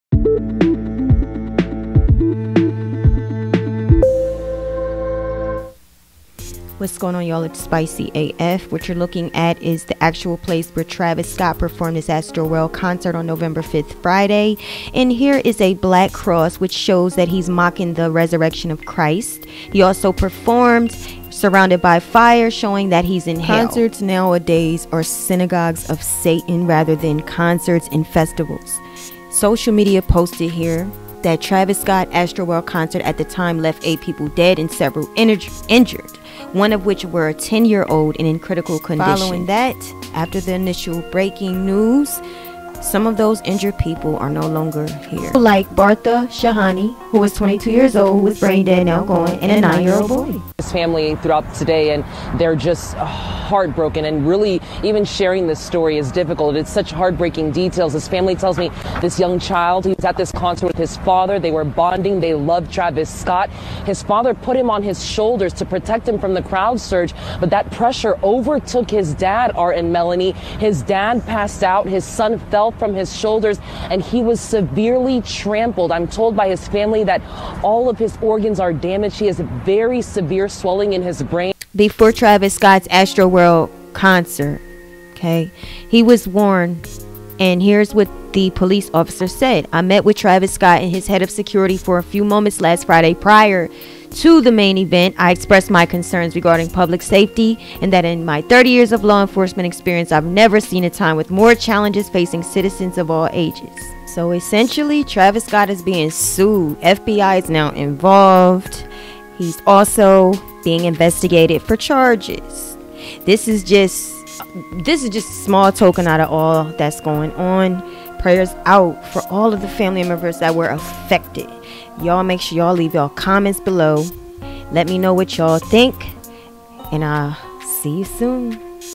Travis Scott's Catastrophic Concert
Travis Scott's Concert was a demonic mating ground leaving a scared, injured and even dead crowd.